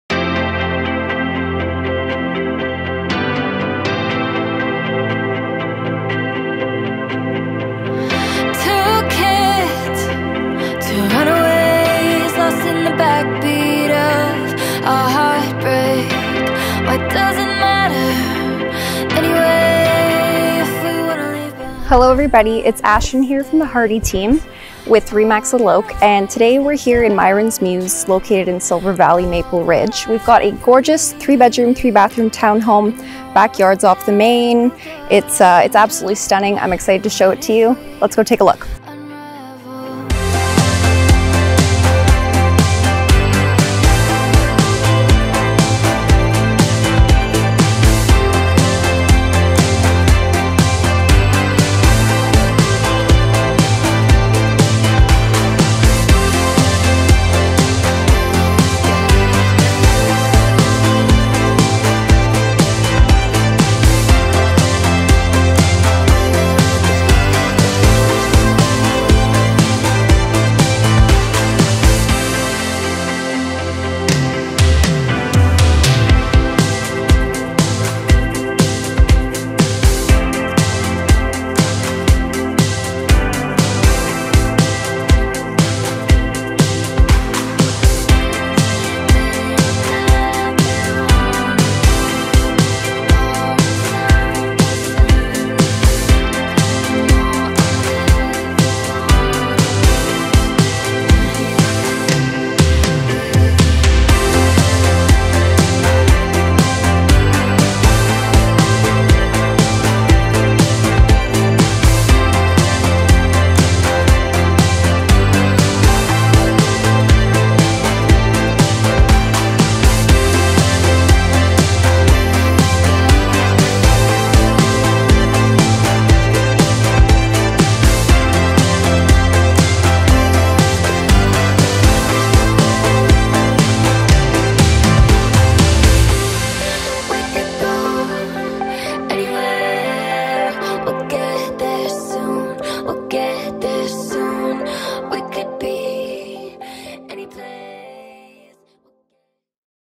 Maple Ridge Townhome at Myron's Muse with Fully Fenced Private Yard, Playground and Community Garden
Your new home search stops here! Immaculate 3 bdrm 3 bath townhome in Myron's Muse, a Silver Valley favourite! Three sizeable bedrooms up and a side-by-side laundry area. The master bdrm features a full ensuite bath, walk-in closet and upgraded shelving. Enjoy your large, fully fenced private backyard off of the kitchen on the main floor. Perfect for entertaining, children, and pets. Gorgeous and bright kitchen including stainless steel appliances, quartz counter tops and ample storage! Enjoy the community garden and playground within Myron's Muse and surrounding views, nature and trails! 10 minutes from everything you need including grocery stores, Golden Ears, dog parks and school. Pet friendly and rentals allowed! Book your private showing today!